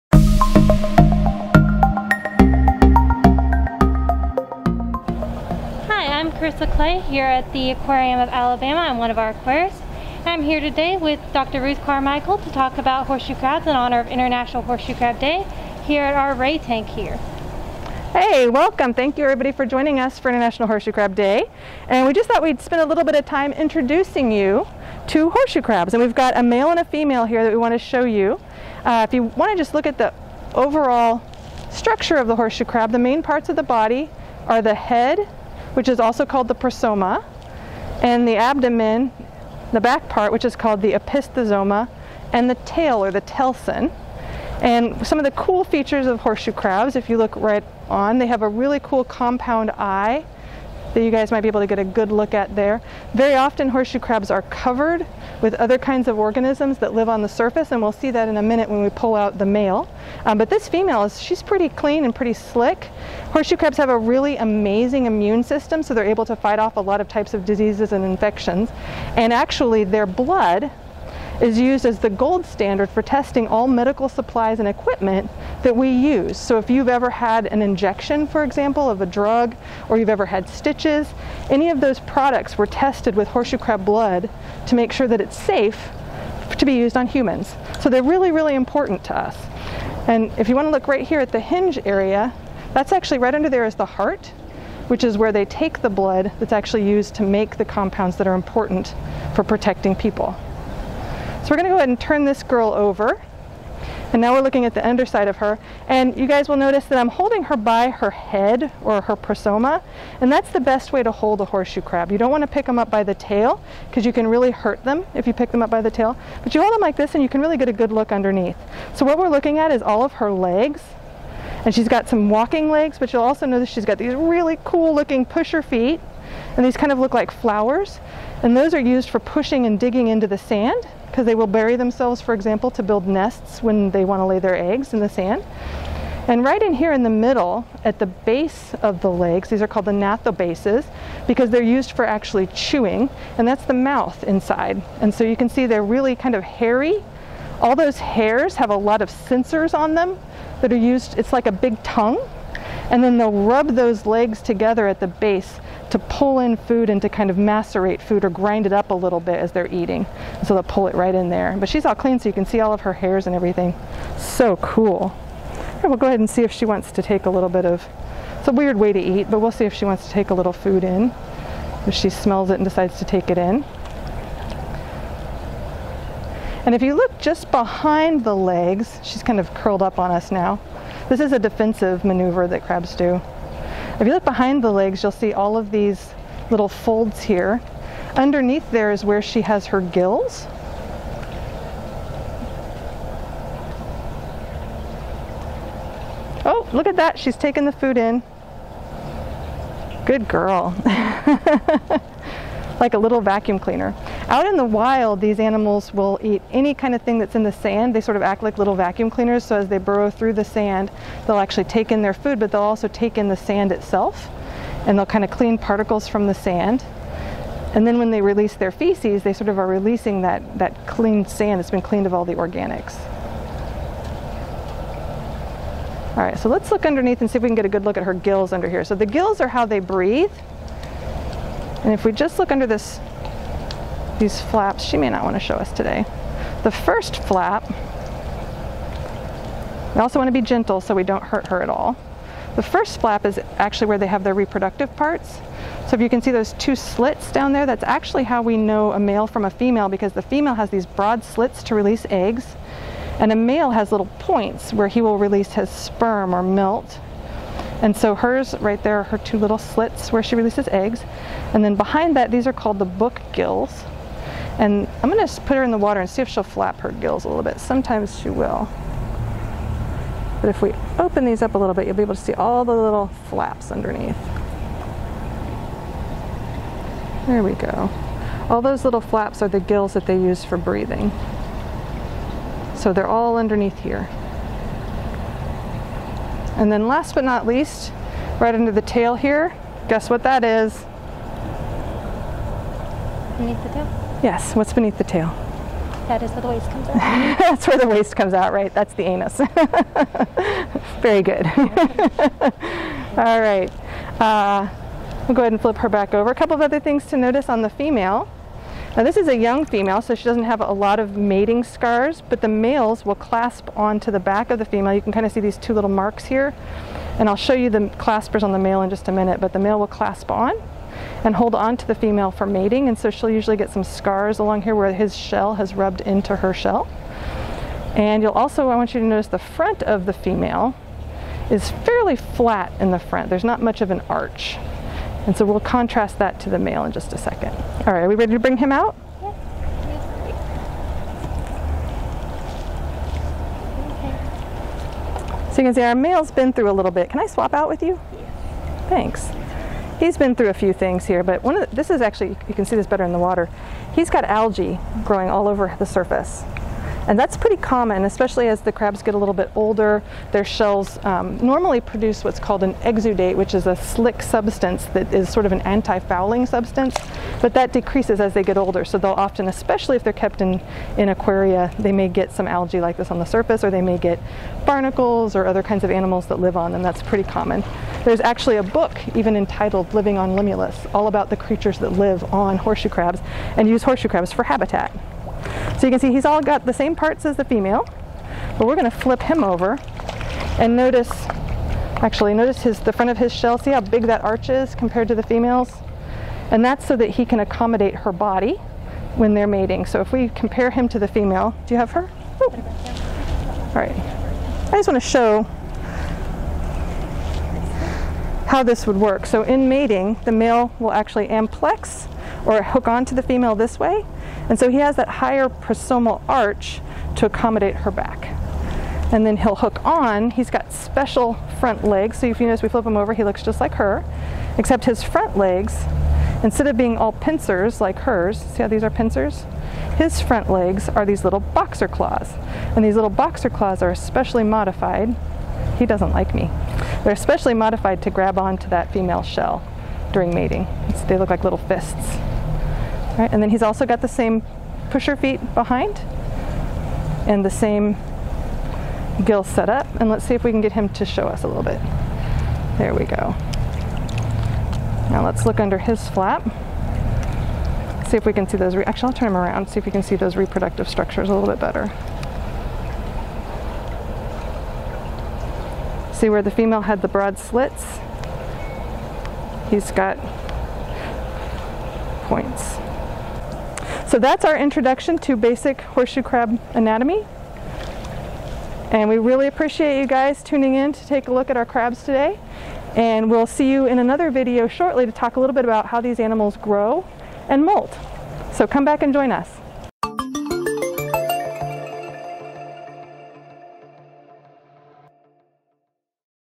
Horseshoe Crab Anatomy
Dr. Ruth H. Carmichael, Dauphin Island Sea Lab and the University of South Alabama, explains the differences and similarities between the male and female horseshoe crabs.